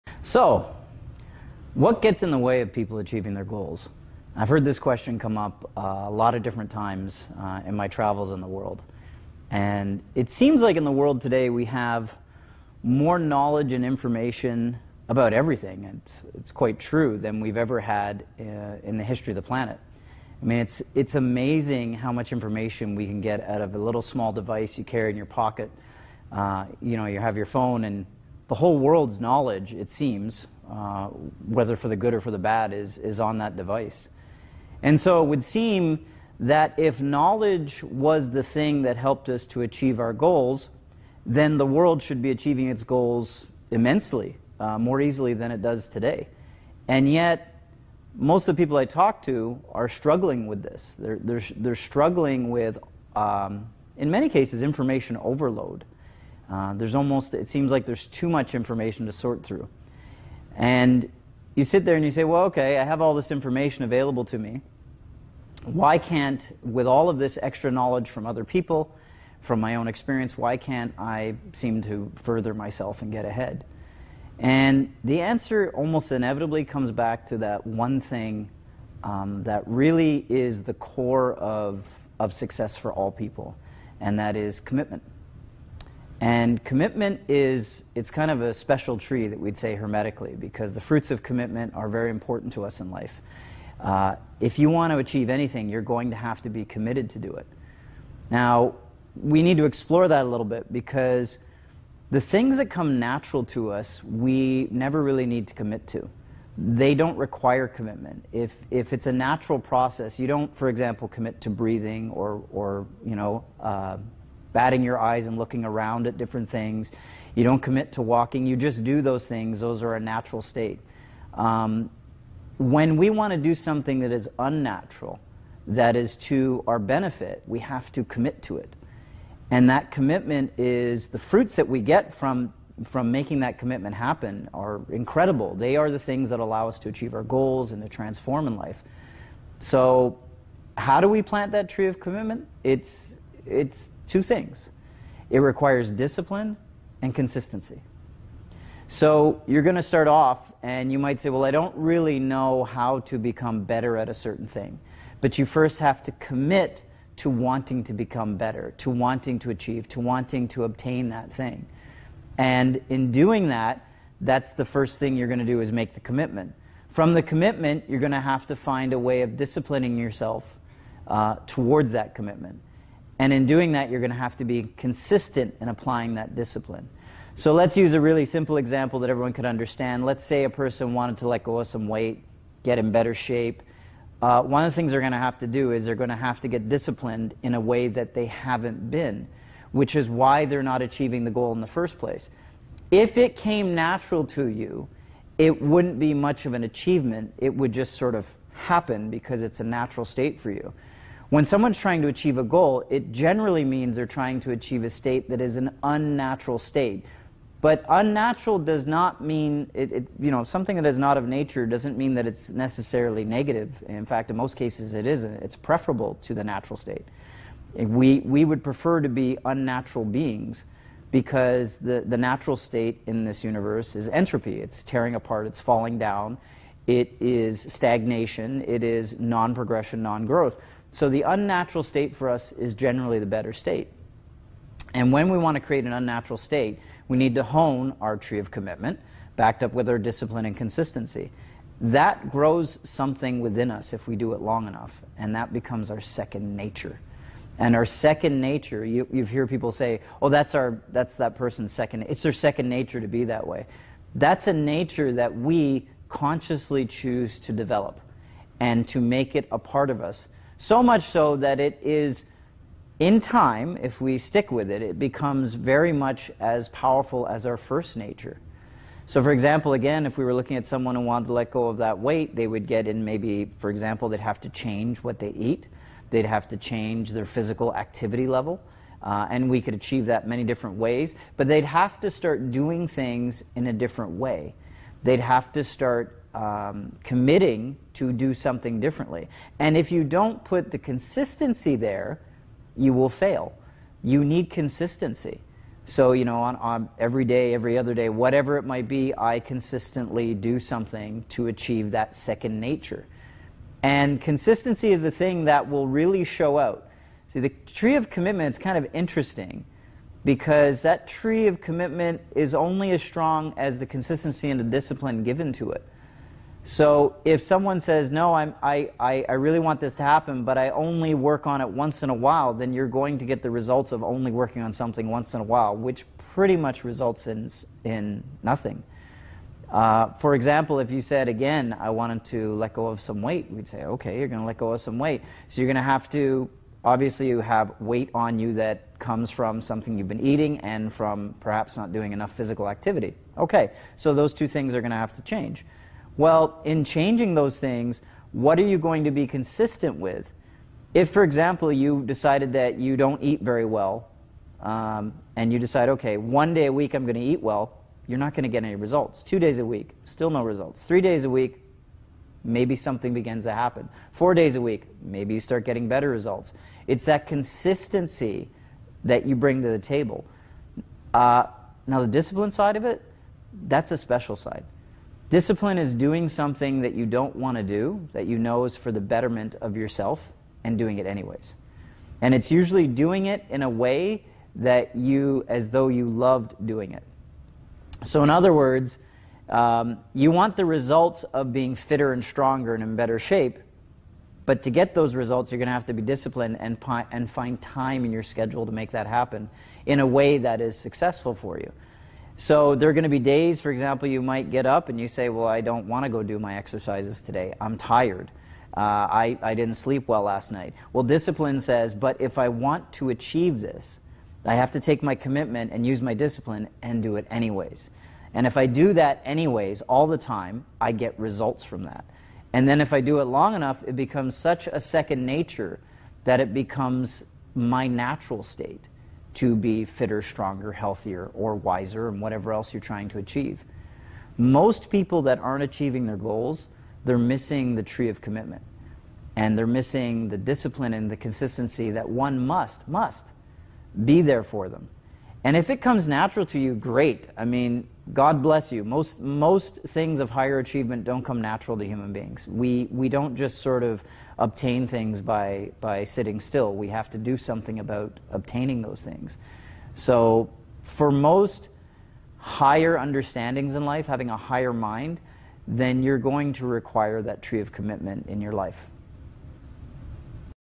Achieving Your Goals with Dave Lanyon
Hermetic teacher, Dave Lanyon, talks with you about some of the things that get in the way of achieving your goals and what to do about it.